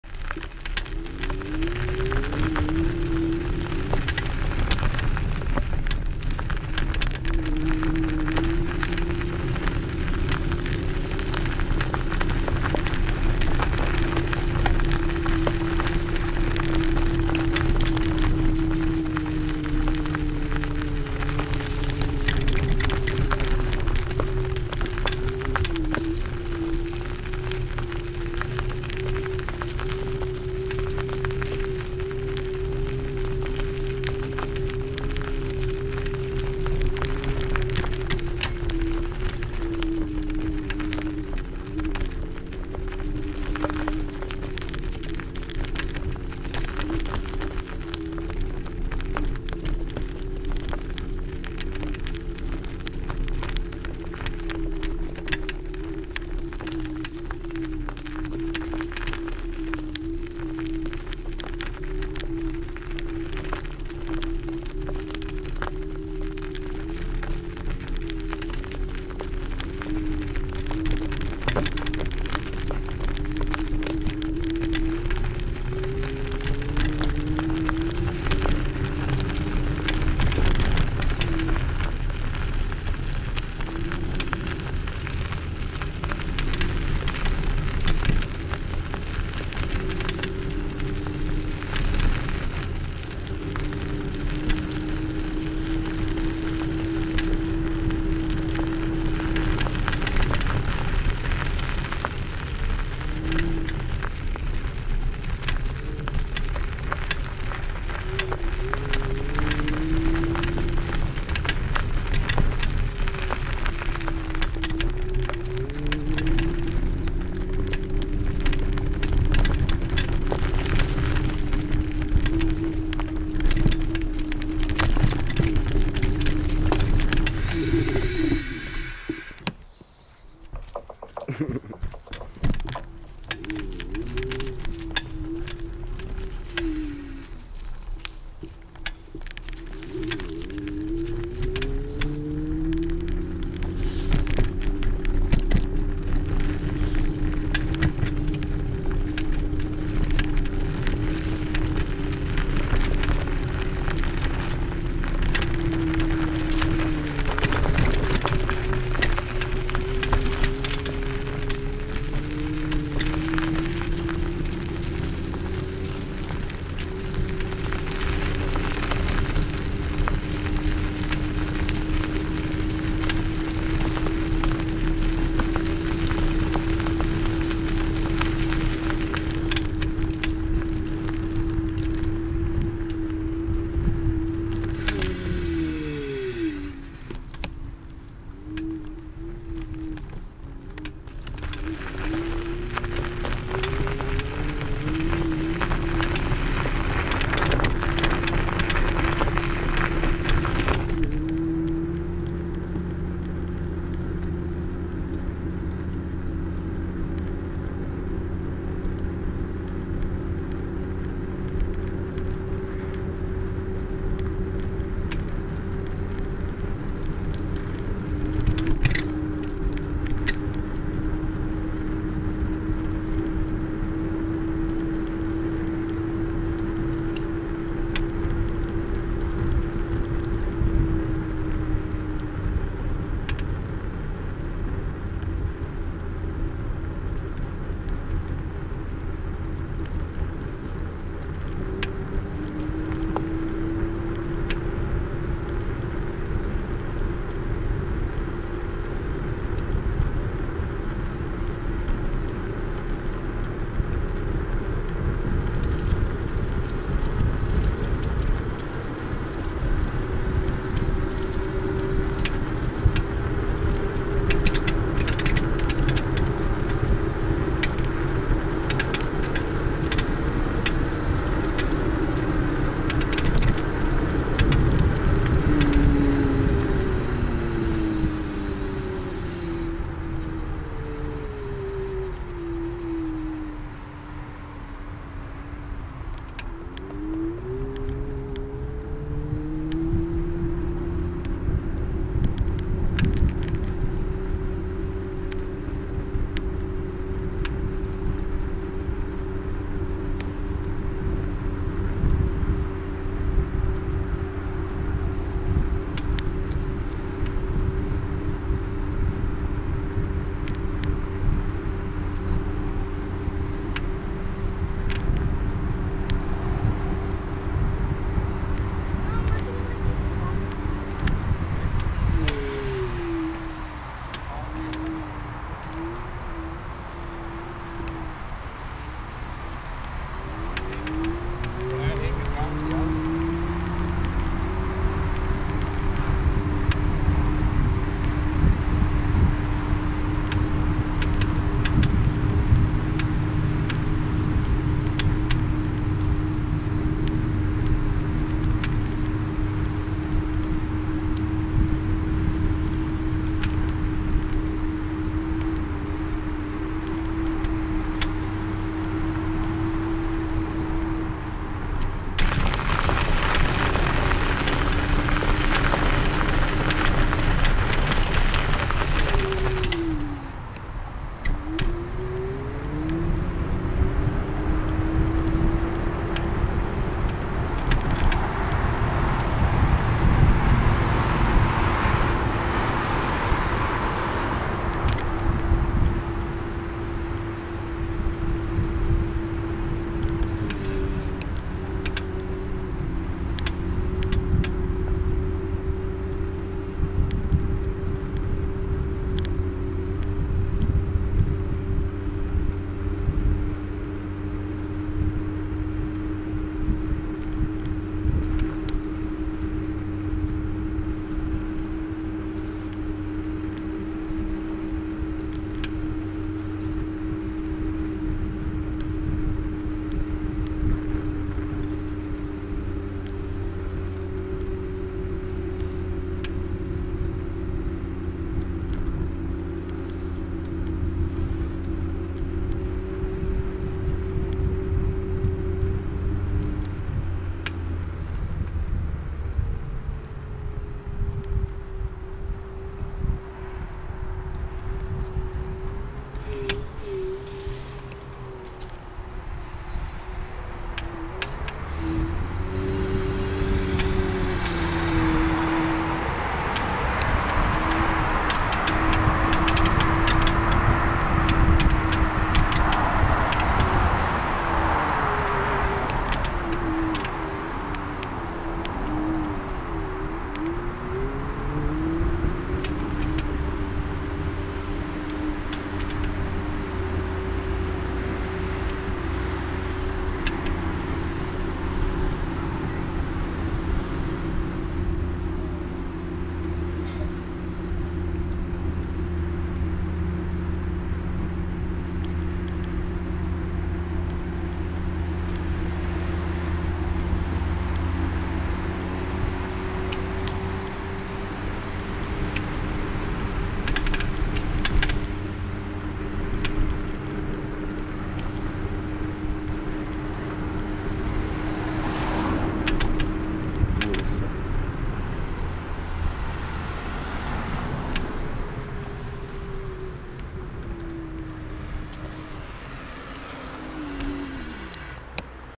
Test tour in forest with Segway Ninebot Max G30D in 4K Bern Zentrum Paul Klee
A test drive on the beginning goes up !